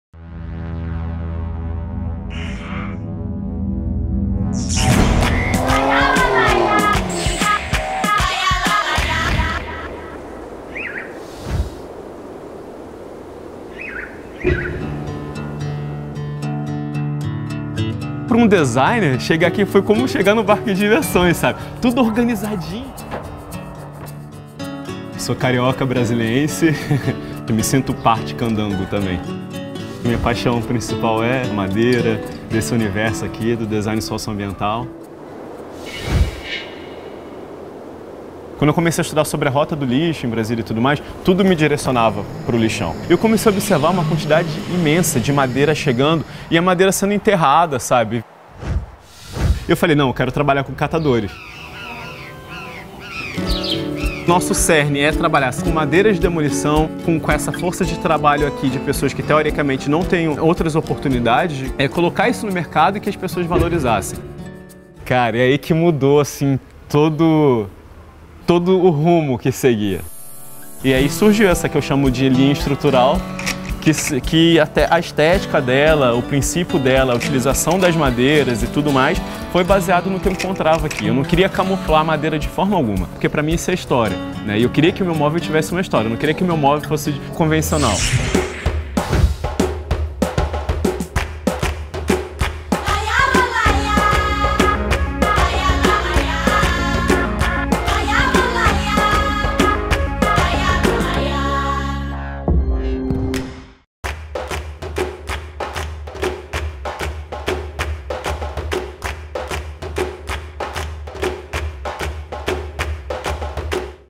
Nissan | Rio 2016 | Rota da Tocha - Móveis com História
Quem se atreve a buscar inspiração no lixo para desenvolver sua primeira coleção de móveis?